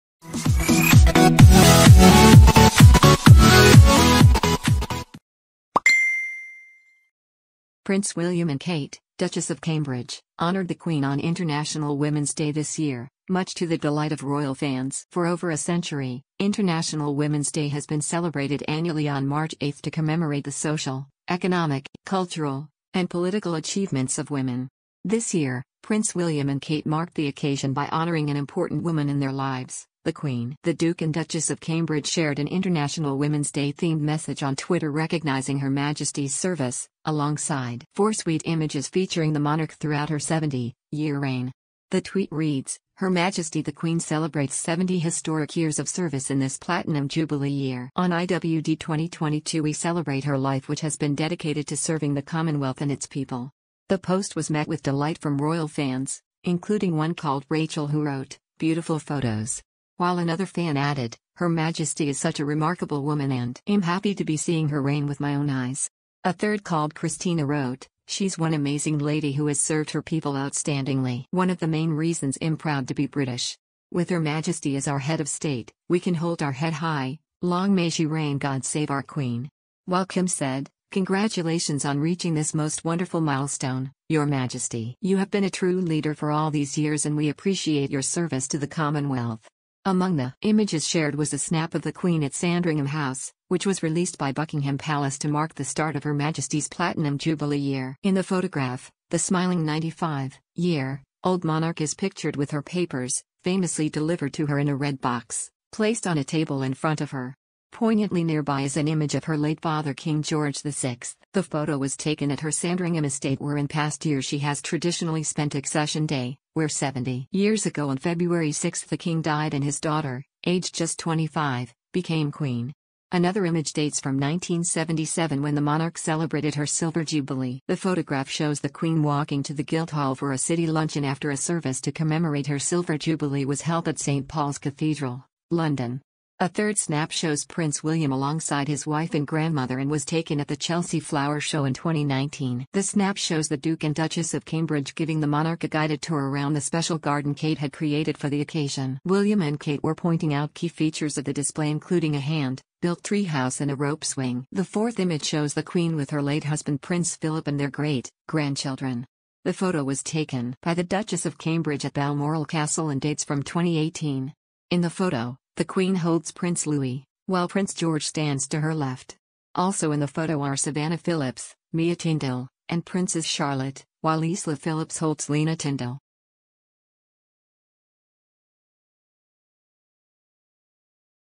'Beautiful photos' Kate and William spark royal frenzy as they honour 'dedicated' Queen
PRINCE William and Kate, Duchess of Cambridge, honoured the Queen on International Women's Day this year, much to the delight of royal fans. For over a century, International Women's Day has been celebrated annually on March 8 to commemorate the social, economic, cultural,...